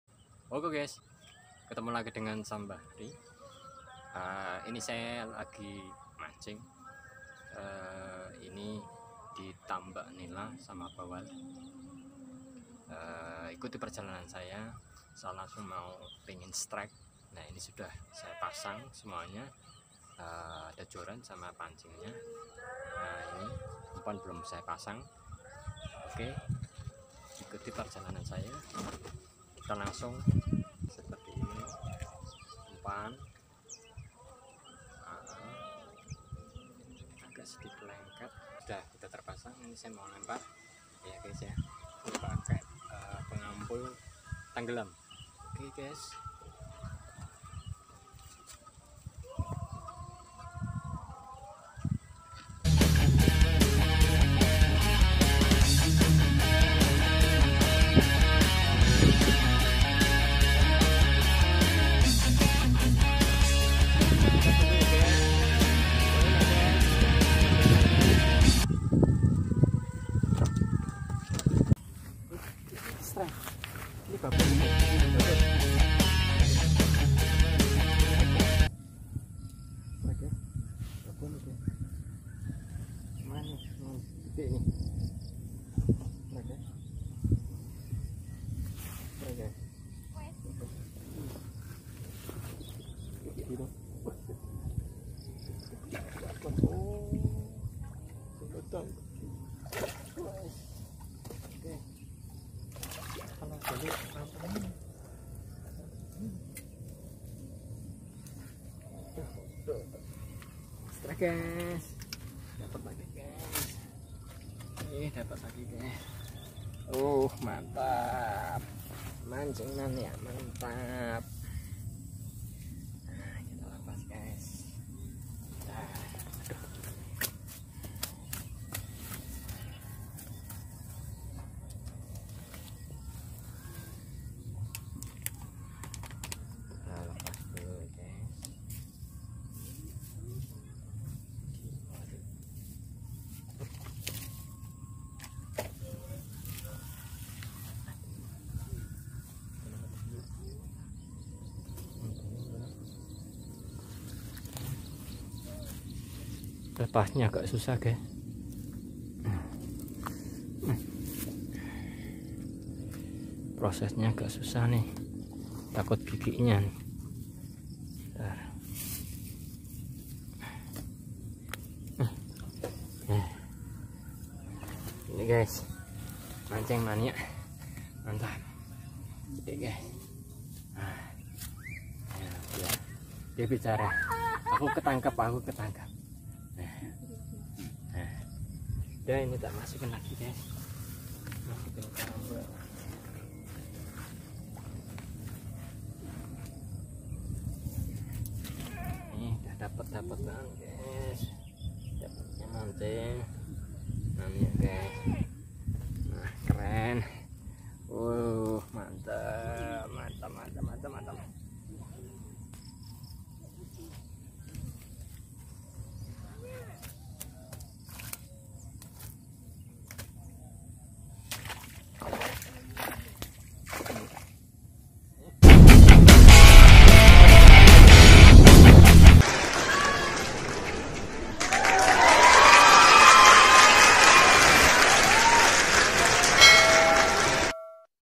Mancing Mania Strike Sambaran Fish Bawal Woow Mantab 👍😀
Hati senang strike disambar ikan Awal Woow Mantab Pokoknya🕺😀